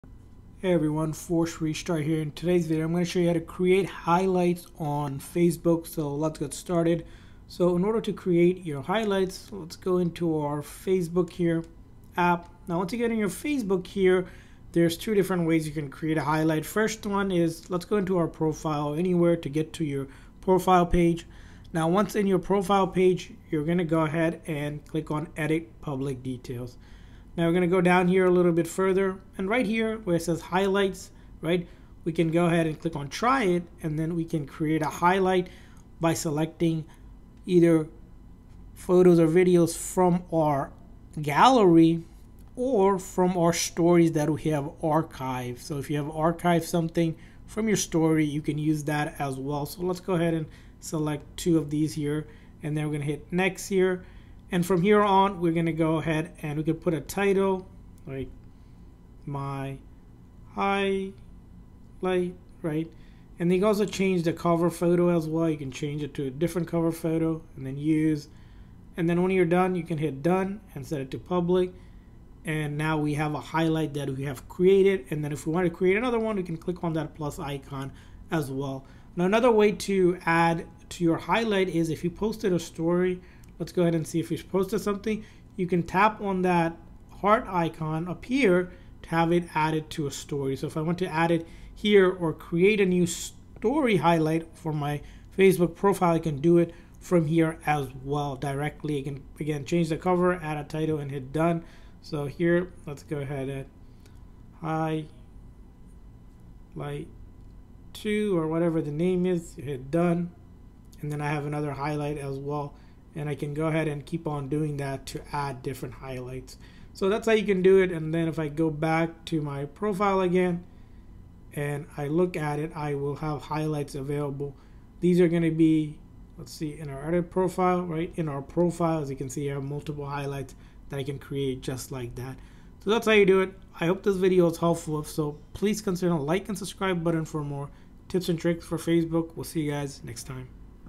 How To Create Highlights On Facebook
In this Facebook tutorial, you will learn how to make story highlights on your Facebook profile page.